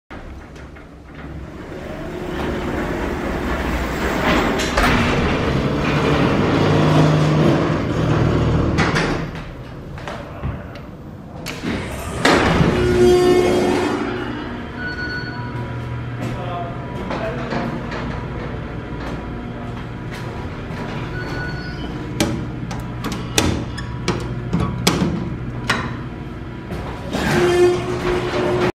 Vintage Montgomery Traction Freight Elevator @ Sears (CLOSED), Woodfield Mall, Schaumburg, IL
Filmed with Canon SX740 on August 12, 2021

This is the original freight elevator in the Sears at Woodfield Mall. This is a cool 1970s Montgomery with an epic motor.

This store closed permanently on November 14, 2021.

Floors served: 2
Manufacturer: Montgomery
Type: Geared traction freight elevator
Fixtures: Square buttons
Capacity: 4000 lb
Year installed: 1971
Illinois ID number: T006383

Location: Sears, 2 Woodfield Mall, Schaumburg, Cook County, Illinois, United States.